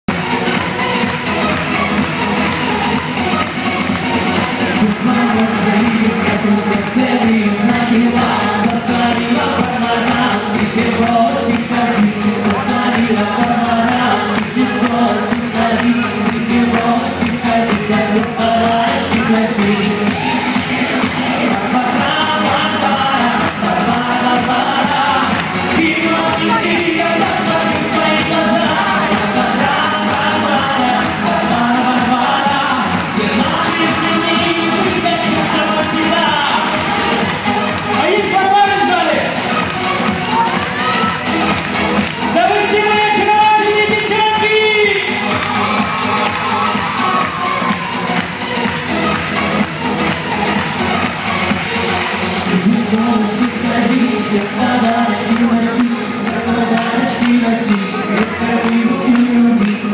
АРТЁМ РАЙЗМАН и Образцовый ансамбль танца ВДОХНОВЕНИЕ
Артём Райзман. Певец, музыкант, ведущий официальных мероприятий, конферансье, шоумен.
и Образцовый ансамбль танца "ВДОХНОВЕНИЕ"